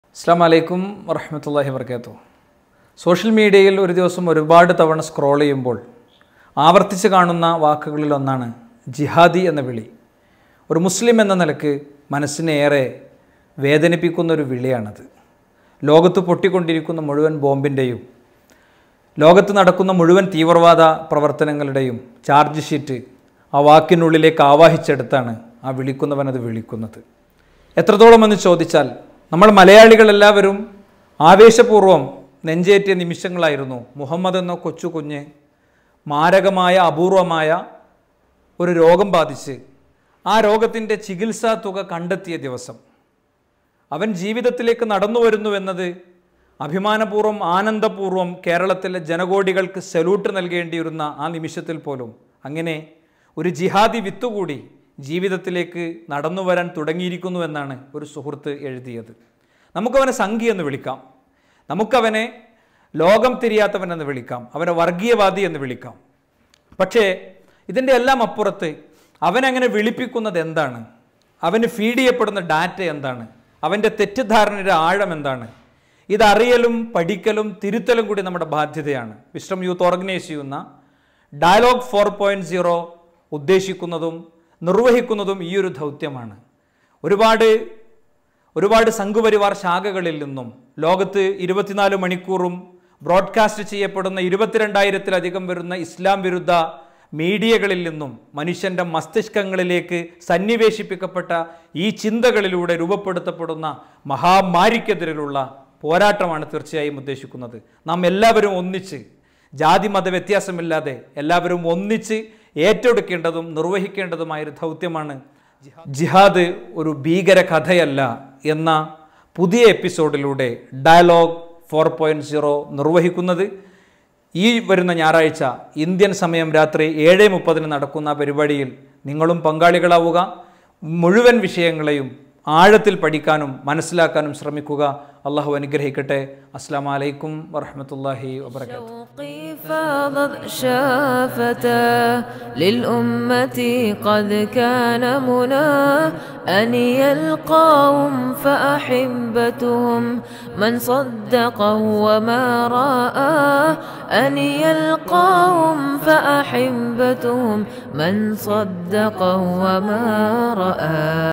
18 കോടി പിരിഞ്ഞത്‌ അവർക്ക്‌ രസിച്ചില്ല;അവനും കിട്ടി 'ജിഹാദീ' വിളി..! | Thajudheen Swalahi
18 കോടി പിരിഞ്ഞത്‌
അവർക്ക്‌ രസിച്ചില്ല;അവനും കിട്ടി 'ജിഹാദീ' വിളി..!

ഭീകര പ്രസ്ഥാനമായ ISIS ലേക്ക് കേരളത്തിൽ നിന്നും റിക്രൂട്ട്മെൻ്റ് നടക്കുന്നുണ്ടോ.?

ജിഹാദ് ഒരു ഭീകര കഥയല്ല
Dialogue 4.0
ജൂലൈ 11 ഞായർ
ഇന്ത്യൻ സമയം രാത്രി 7:30 ന്

Live on @wisdom youth fb page & wisdom global tv youtube channel